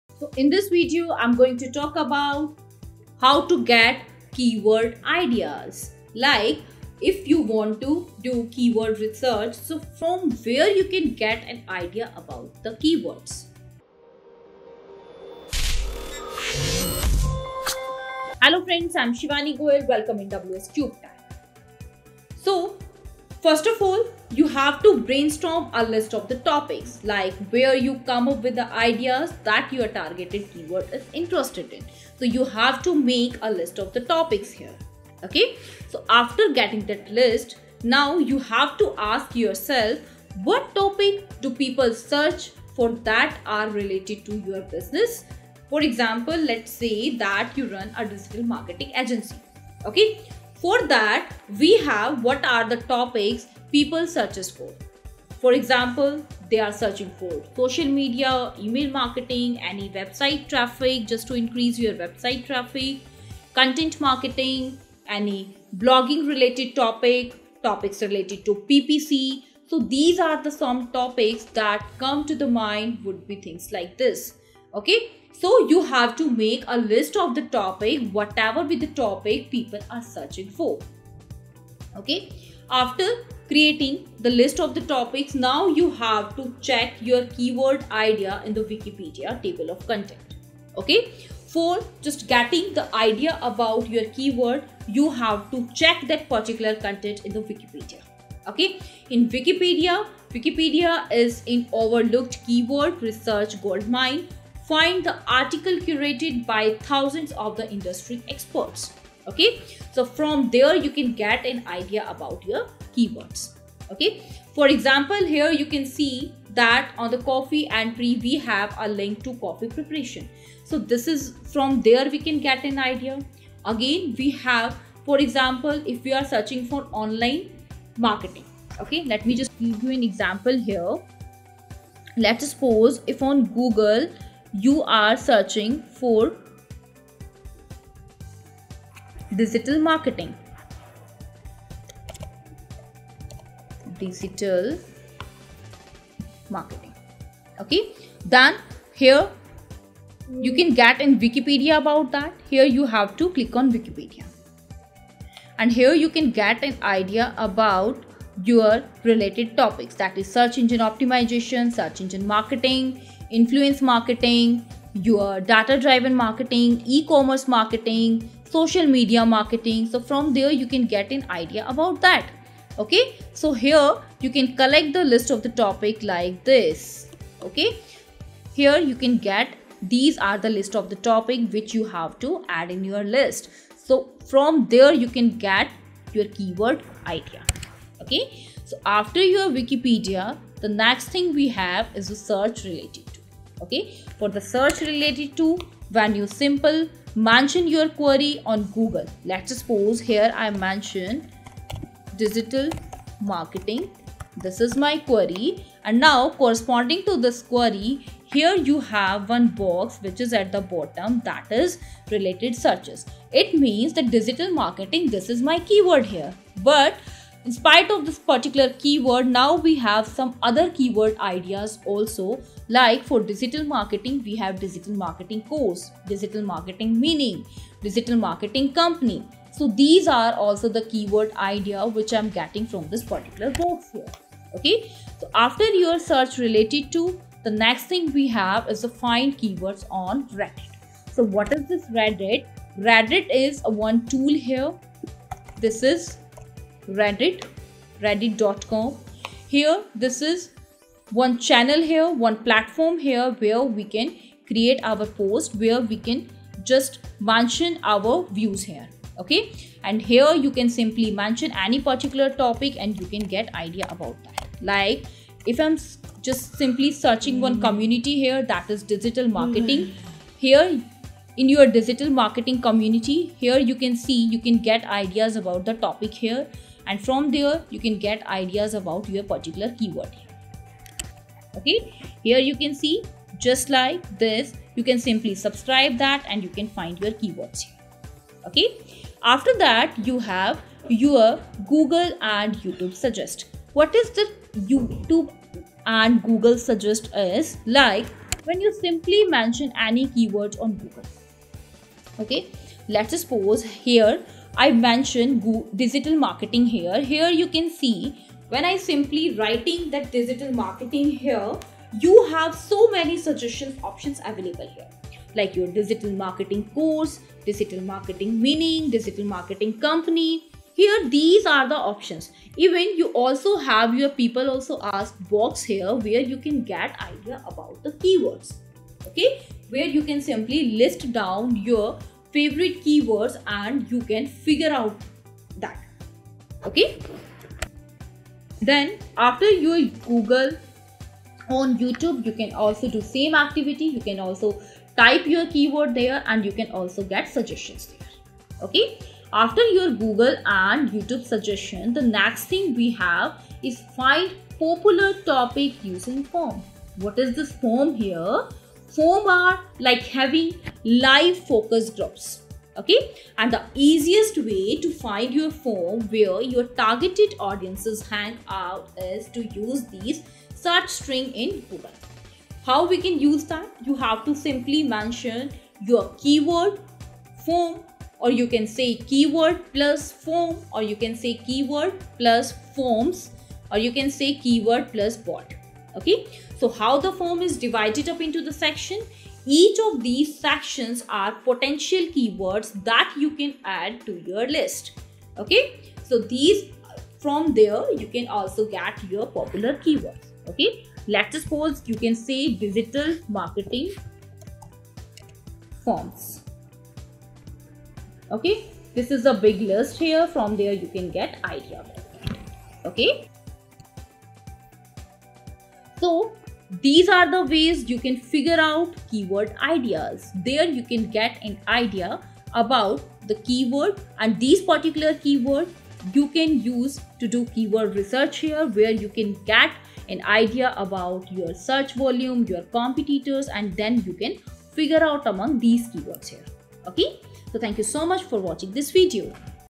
SEO Tutorial - How to find Keyword Ideas Complete Guide In English
In this video, SEO Tutorial - How to find Keyword Ideas Complete Guide In English. Find all the videos of the Complete SEO Tutorials in English Course in this playlist